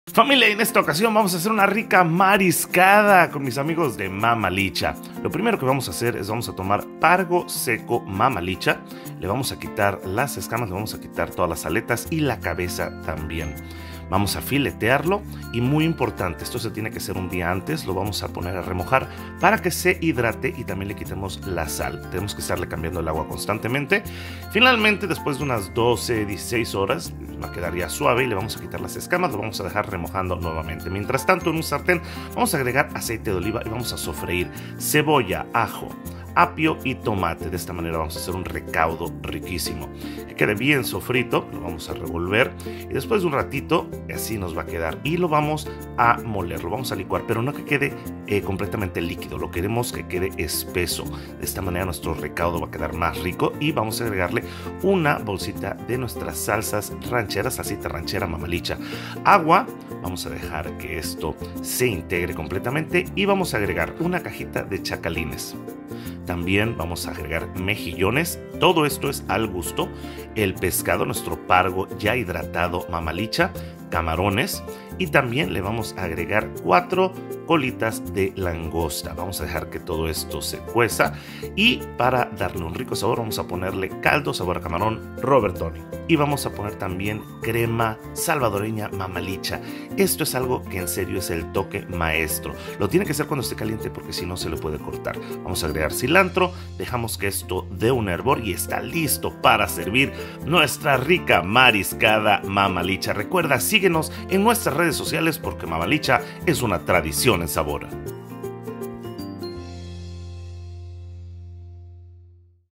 Cómo hacer sopa de mariscos salvadoreña- Recetas de Mamá Lycha
La Mariscada se define como un platillo con diferentes combinaciones de comida de mar, pero la de Mamá Lucha tiene  un toque único y diferente que es la crema salvadorena, hace toda la diferencia. ¿Con qué tipo de mariscos te gusta preparar tu sopa de mariscos? Comentanos!

Salvadoran Seafood soup is a dish with different combinations of seafood, what makes ours truly unique is the salvadoran-style cream that adds a unique flavor and texture to the soup. What other seafood do you enjoy your soup with?